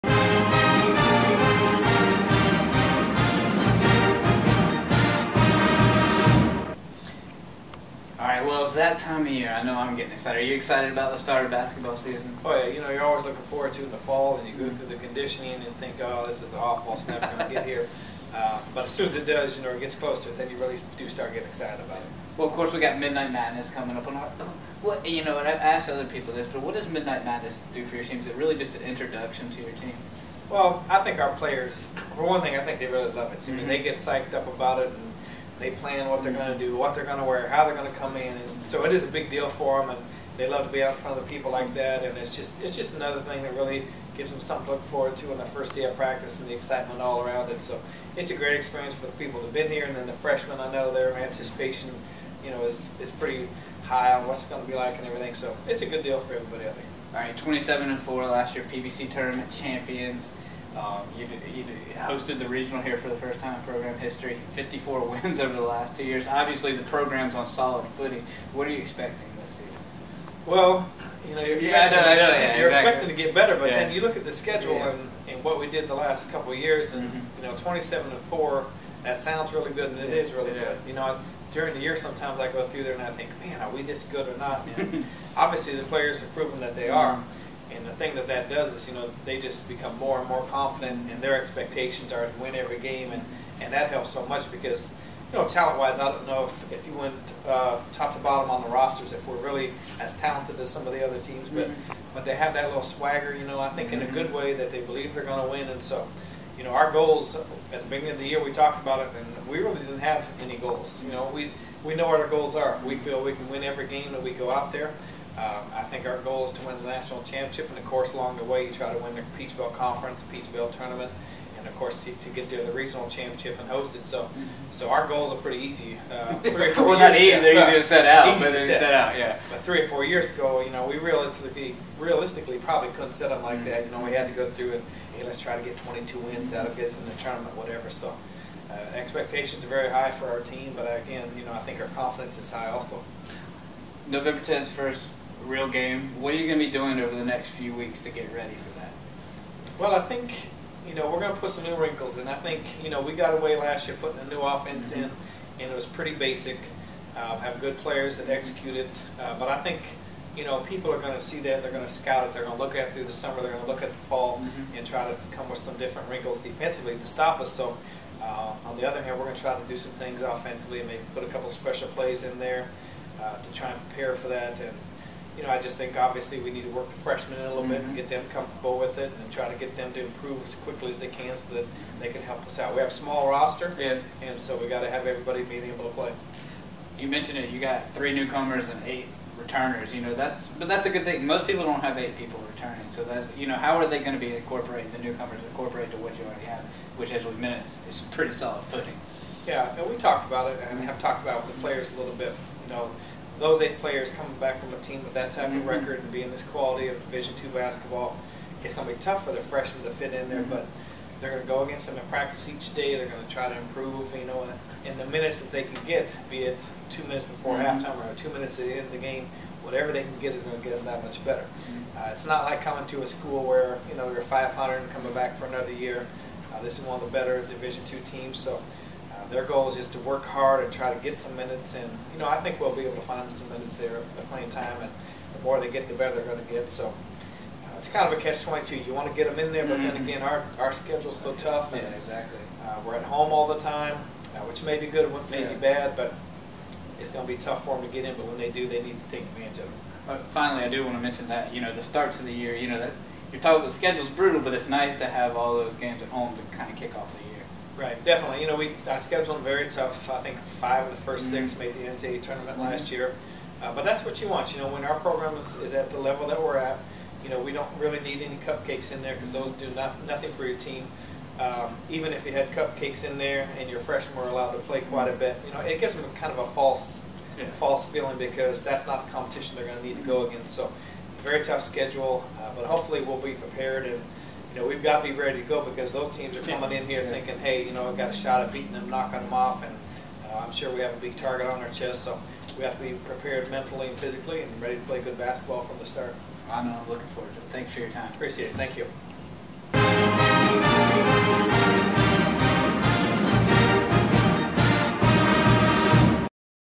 USC Aiken 2012 Women's Basketball Preseason Preview
Assistant AD Brian Hand sits down with USC Aiken's women's basketball head coach Mike Brandt to discuss the upcoming season and Midnight Madness.